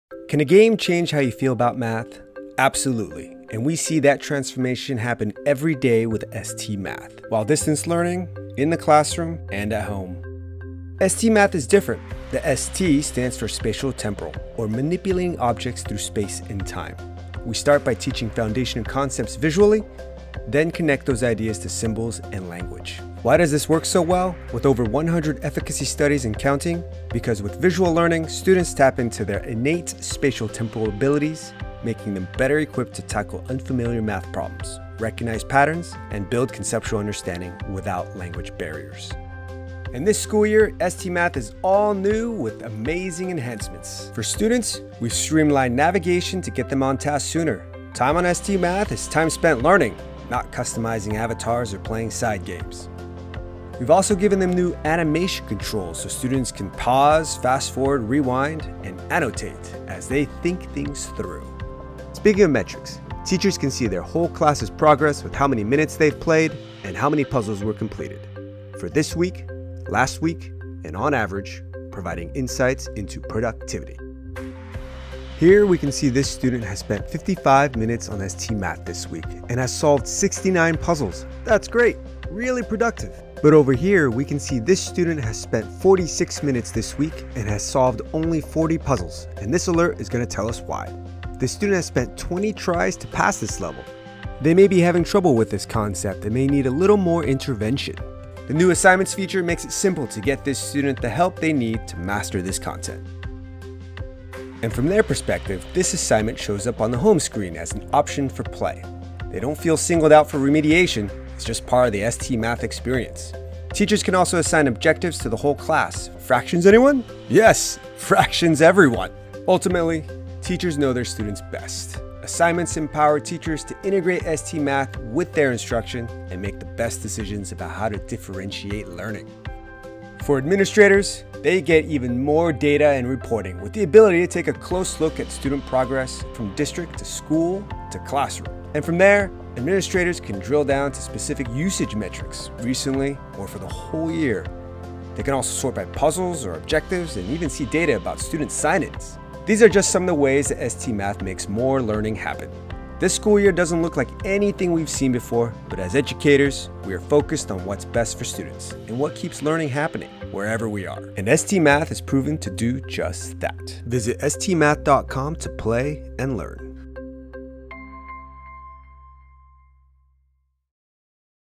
ST Math is All New for 2020-2021 - What Are Some of the New Features?
Take a quick tour of ST Math, which is all new for the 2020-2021 school year, with exciting new features for students, teachers, and administrators. See why ST Math is effective while distance learning, in the classroom, and at home!